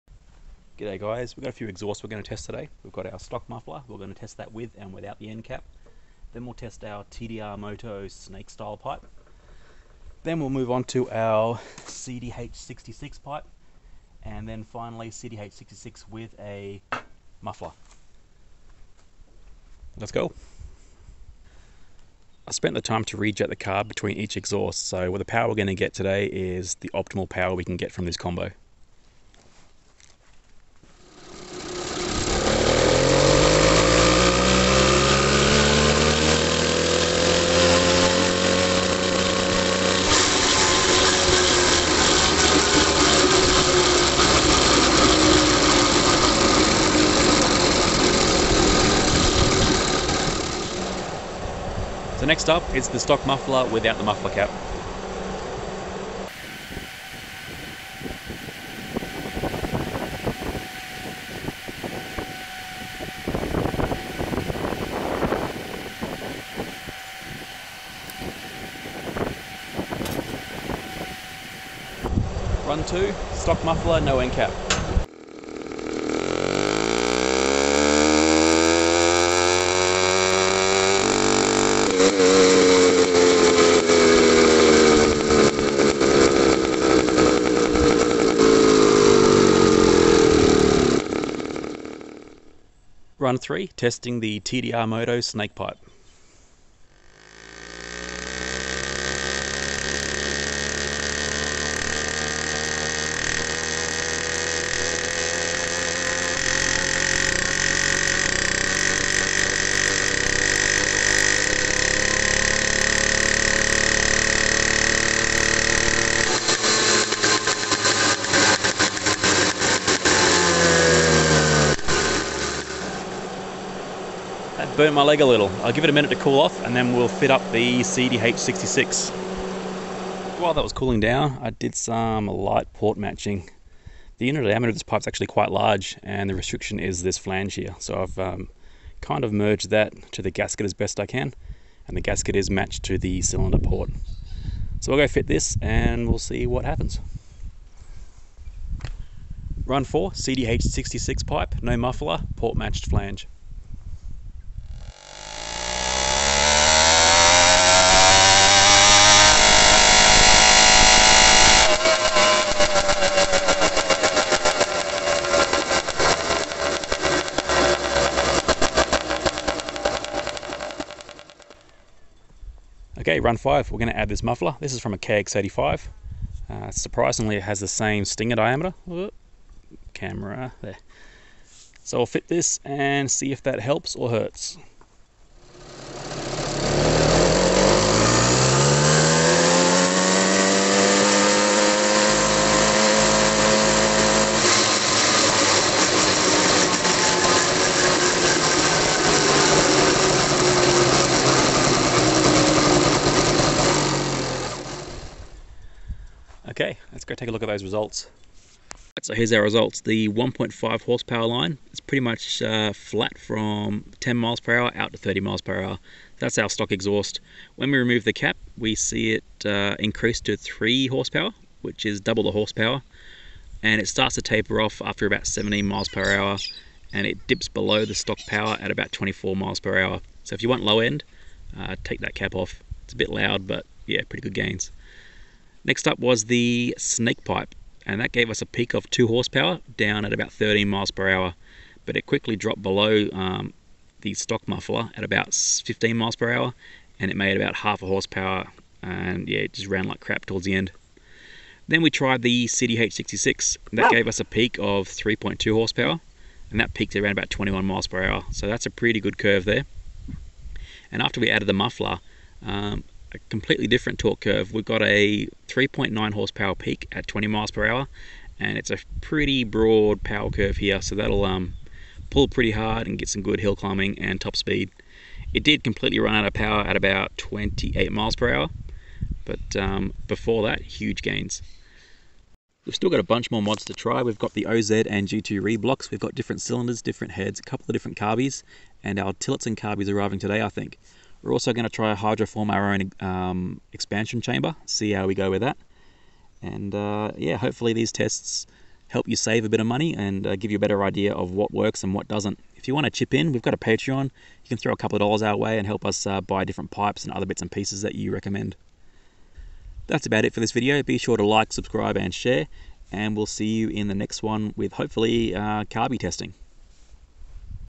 80cc motorized bicycle performance mods - part 2 - Exhaust testing
Part 2 - Aftermarket Exhaust upgrades!

Ever wondered how much HP cheap mods and aftermarket parts can make? Let's find out!

In this episode we'll test out stock exhaust mods, the CDH66 (MZ65 clone) and the TDR Moto 'snake' pipe.